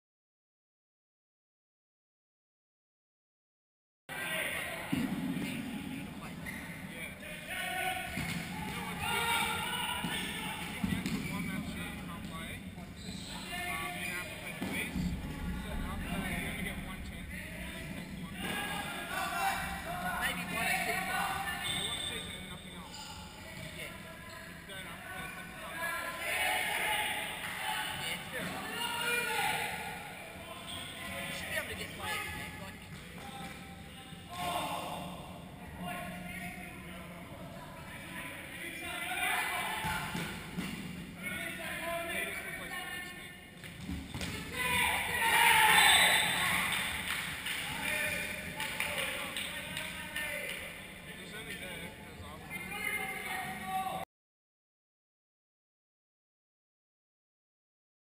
FC/DB: ROUND 7 v Evolution. 18th March 2013. 7-1 loss
Heaviest loss in DB history.